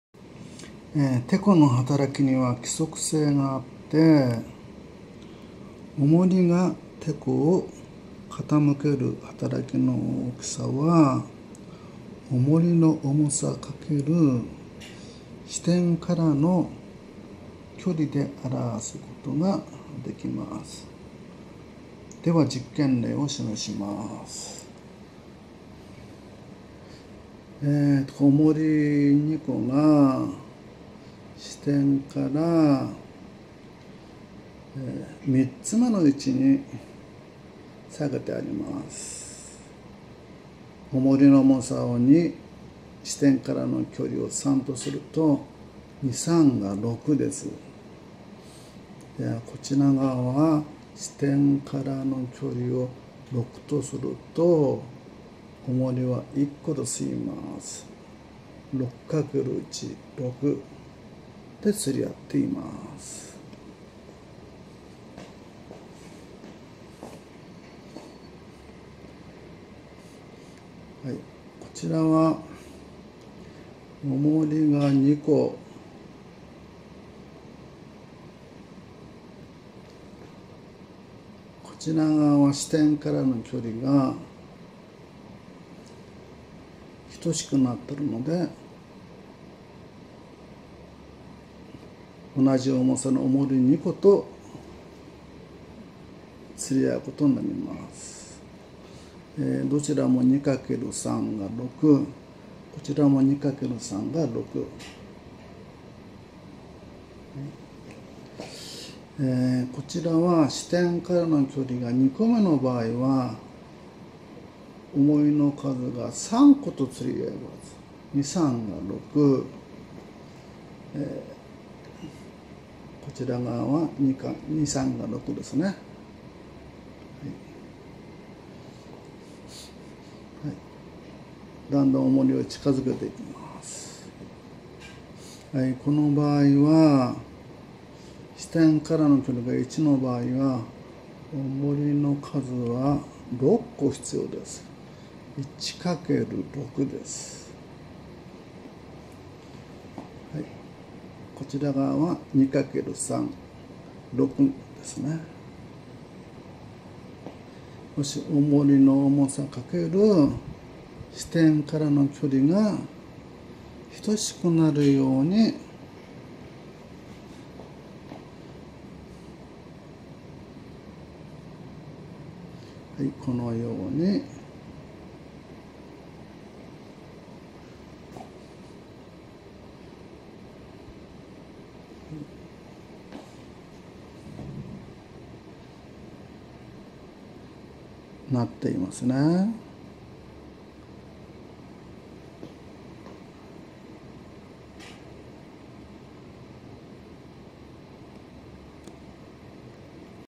てこの原理 小学校６年
てこのはたらきにはきまりがあります。支点からの距離に重さをかけたものがてこを傾けるはたらきの大きさを表わしています。実験の左側で、おもりの重さを２として支点からの距離を３とすると傾けるはたらきの大きさは６になります。右側で支点からの距離を６とするとおもりの重さは１個でつり合うことになります。台秤で力を比較